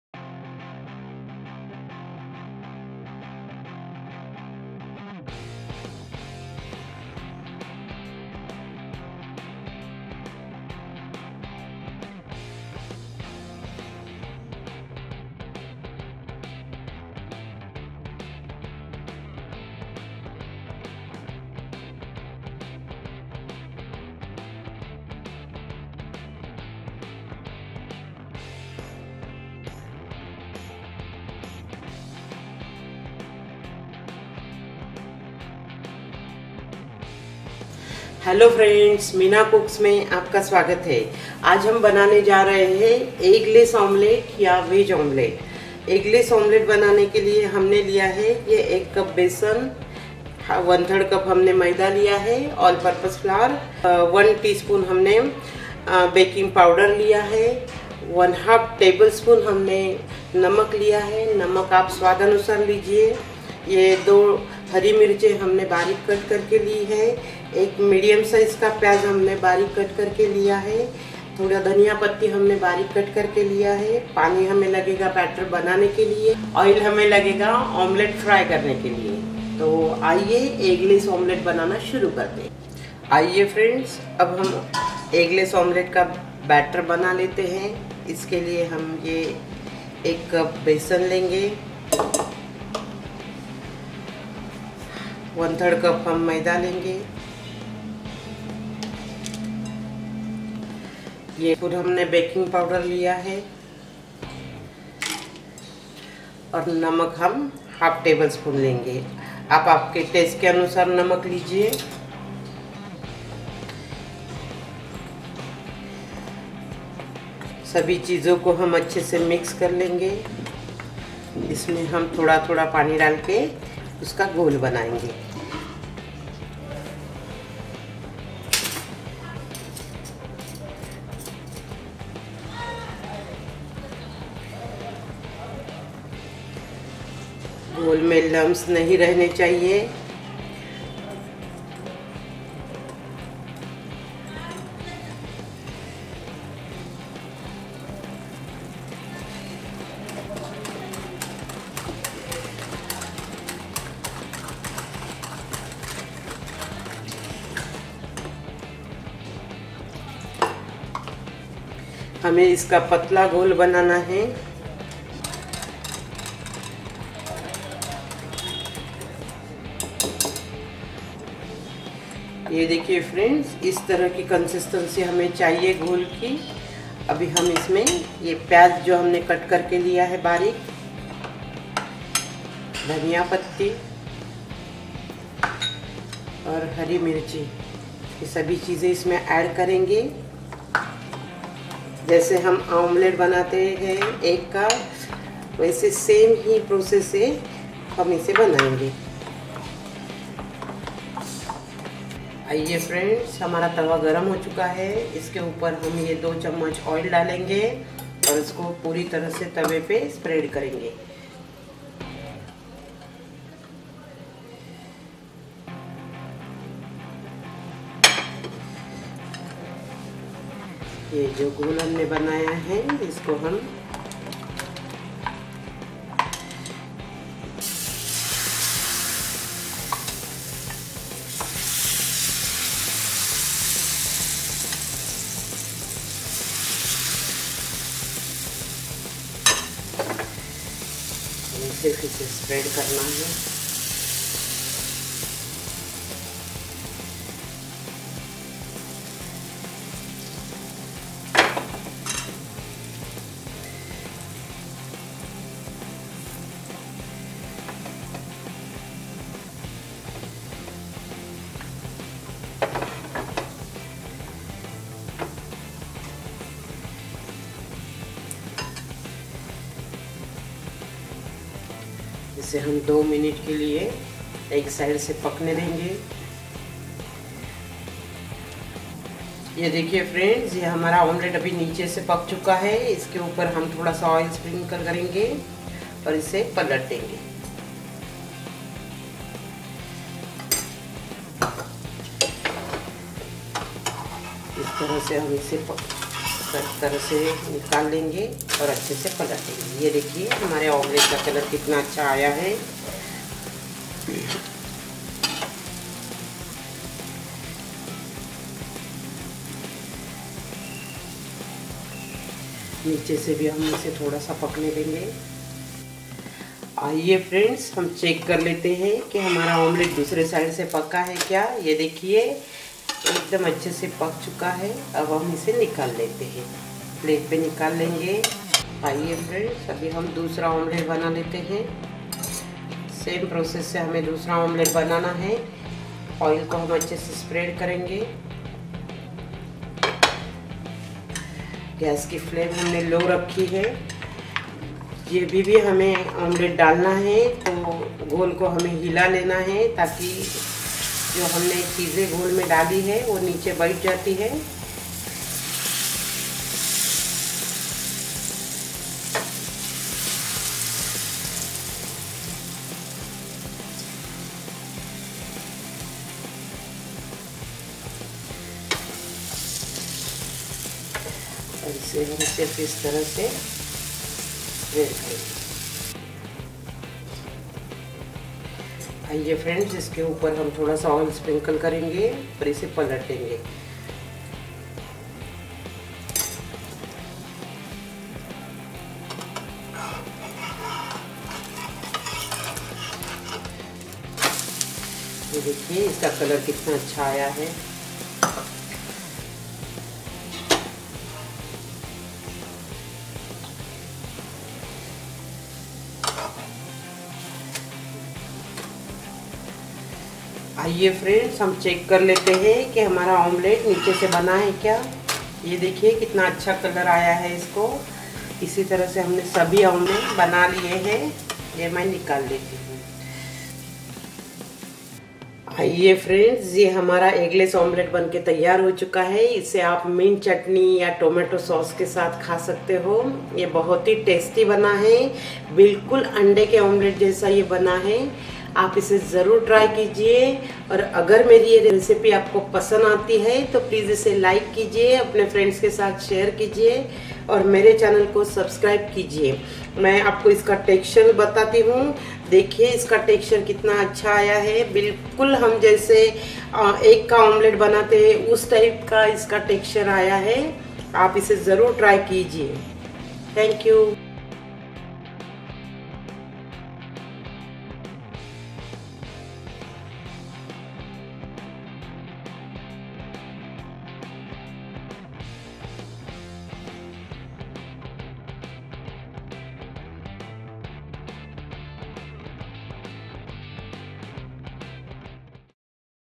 Eggless Omelette अंडे के बिना ऑमलेट कैसे बनाये
Eggless Omelette is very tasty  & easy breakfast recipe by Meena Cooks.